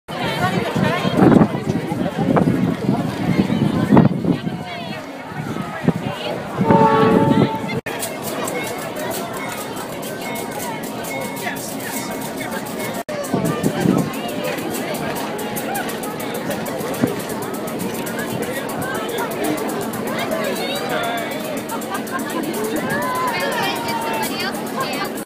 The Easter Bunny arrived in Boyertown yesterday via train.
By: Rebecca Blanchard - Editor of The Boyertown Area Times